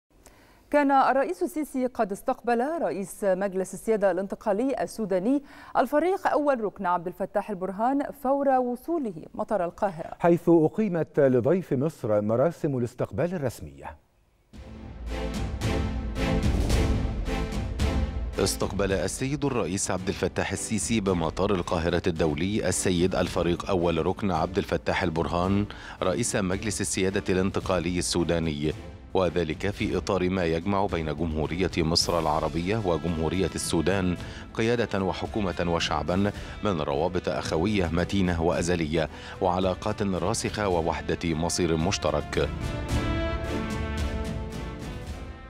نشرة السادسة| متحدث الرئاسة: الرئيس السيسي يستقبل بمطار القاهرة رئيس مجلس السيادة الانتقالي السوداني
نشرة السادسة | متحدث الرئاسة: الرئيس السيسي يستقبل بمطار القاهرة رئيس مجلس السيادة الانتقالي السوداني أول ركن عبد الفتاح البرهان